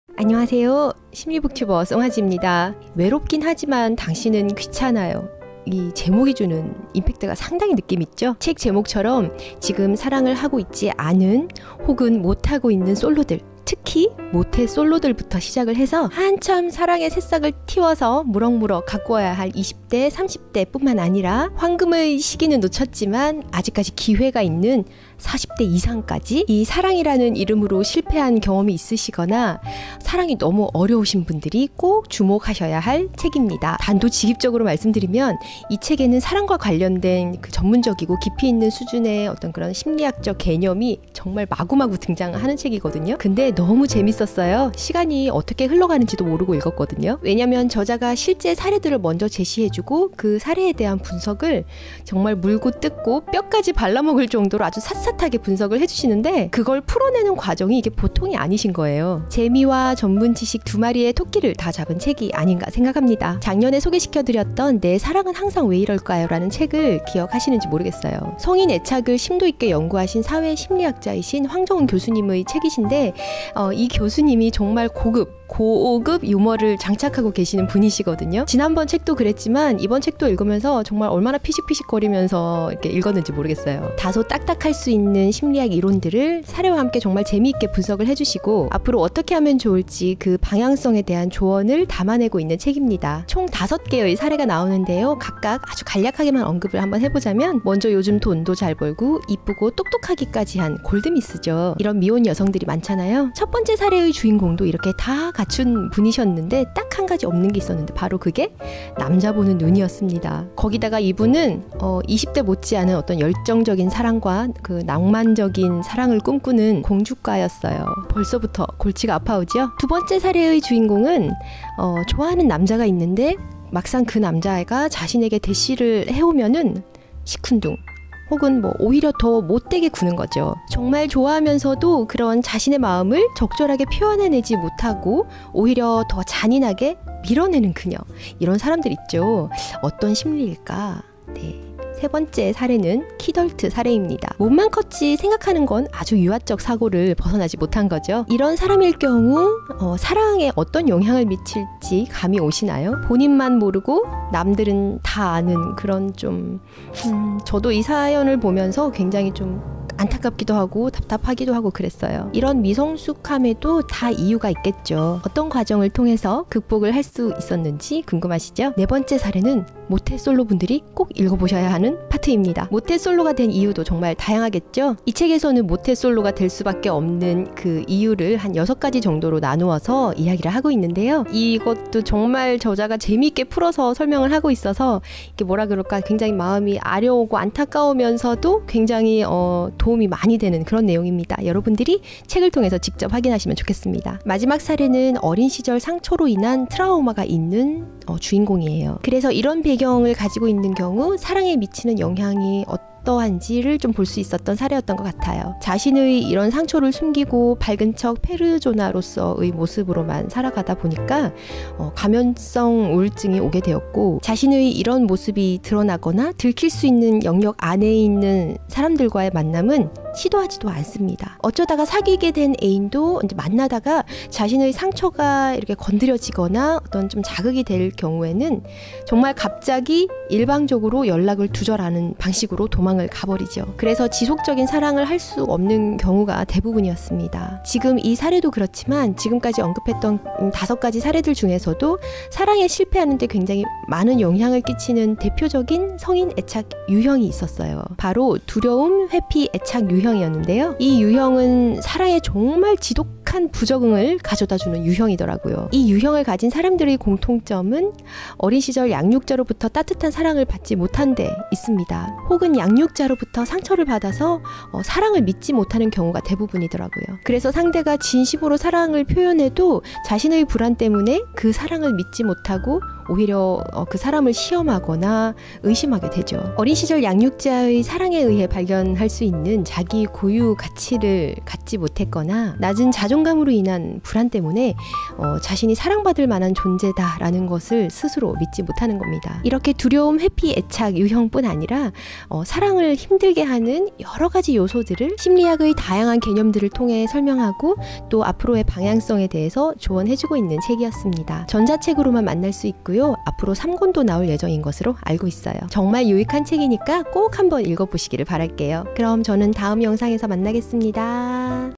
심리북튜버 | 외롭긴 하지만 당신은 귀찮아요 | 사랑을 안하는거지.. 못하는게 아니거든요 ㅡㅡa?!? | 과연..그럴까?? | 당신의 사랑을 심리학자가 분석해 드립니다.
제목 :  외롭긴 하지만 당신은 귀찮아요
저자 :  황정은
출판사 : 브레인푸드

심리북튜버 쏭아지네의 채널에 오시면 더욱 다양한 심리학 관련 서적을 만나실 수 있어요 :-)